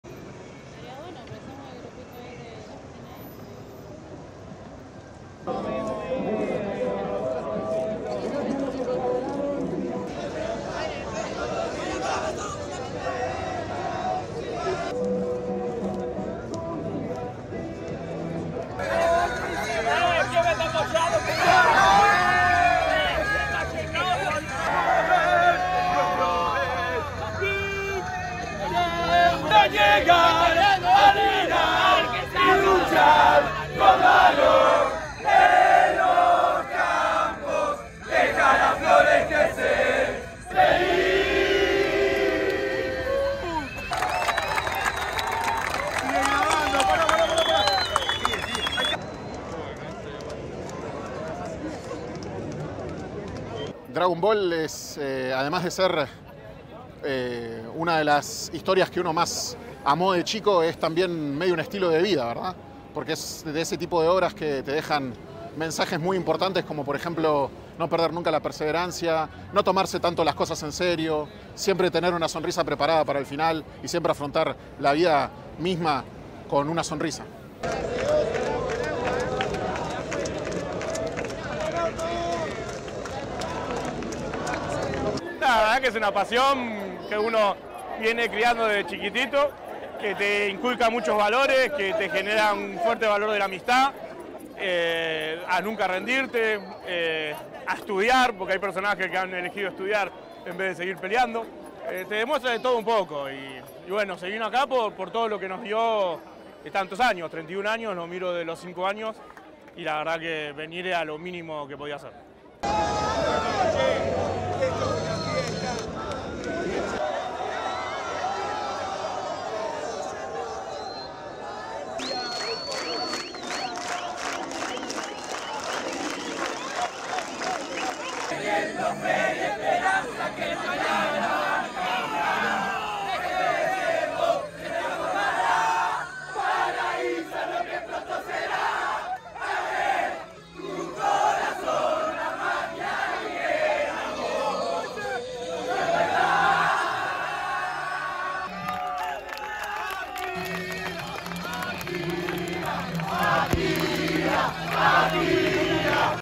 Argentines pay homage to Dragon Ball creator | ABS-CBN News
Argentine fans of Japanese anime Dragon Ball gathered on Sunday (March 10) at Buenos Aires’ iconic obelisk to remember Akira Toriyama, the creator of the manga comic the cartoon was based on.